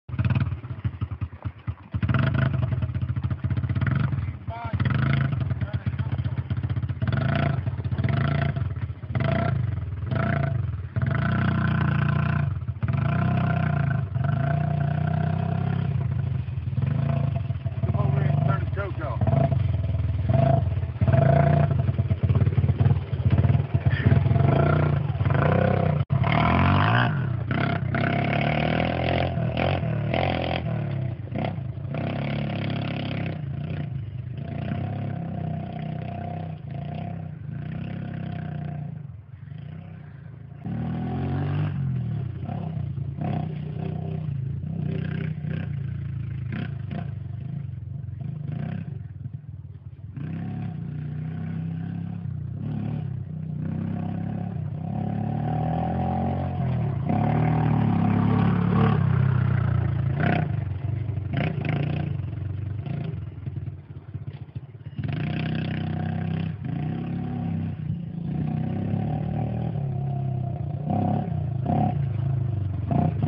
Driving my son's yamaha 80 without prosthetics
Just because you have a disability doesn't mean you can't have fun!